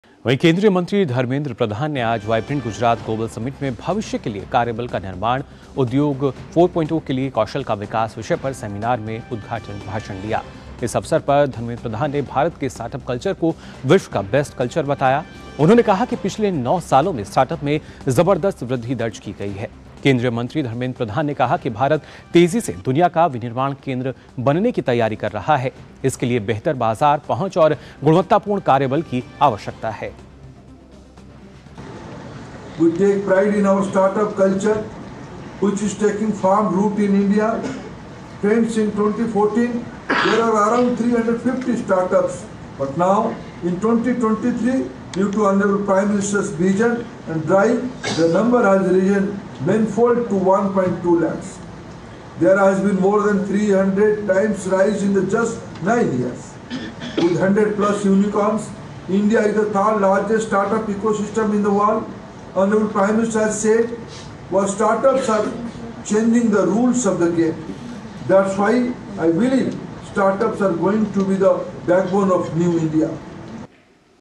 Vibrant Gujarat Global Summit में केंद्रीय मंत्री धर्मेंद्र प्रधान का संबोधन
केंद्रीय मंत्री धर्मेंद्र प्रधान ने आज वाइब्रेंट गुजरात ग्लोबल समिट में भविष्य के लिए कार्यबल का निर्माण: उद्योग 4.0 के लिए कौशल का विकास विषय पर सेमिनार में उद्घाटन भाषण दिया।

वाइब्रेंट गुजरात ग्लोबल समिट आत्मनिर्भर भारत के लिए समृद्ध गुजरात की परिकल्पना के साथ नई ऊंचाइयां हासिल करता रहेगा। वहीं, गुजरात के सीएम का कहना है कि समिट हमारे प्रधानमंत्री की परिकल्पना के अनुसार विकास और सामाजिक-आर्थिक वृद्धि के प्रति हमारी प्रतिबद्धता का प्रतीक है।